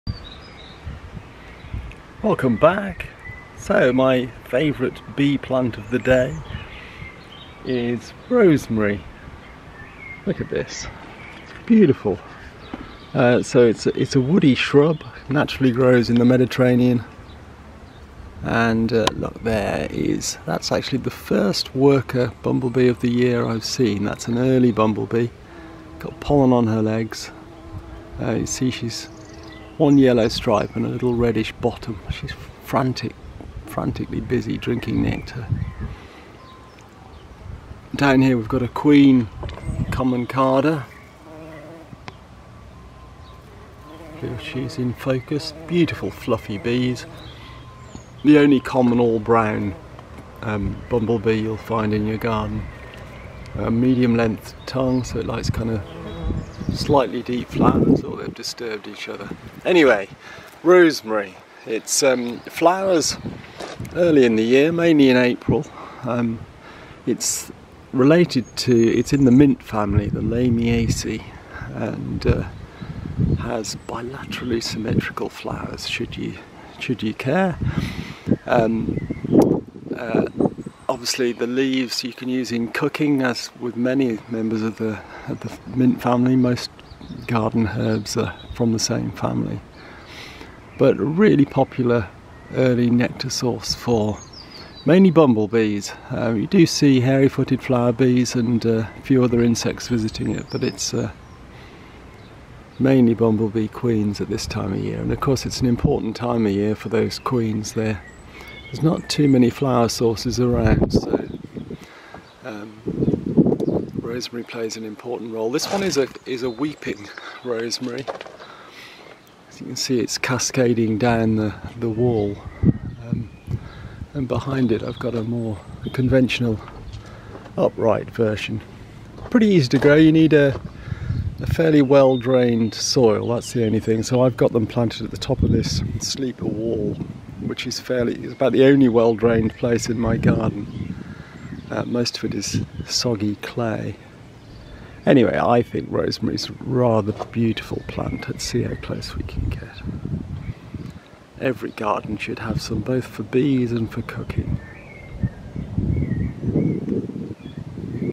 Some of my favourite garden flowers for bees: Rosemary
One of a series of short videos highlighting some of the more beautiful and bee-tastic flowers that you might like to grow in your garden.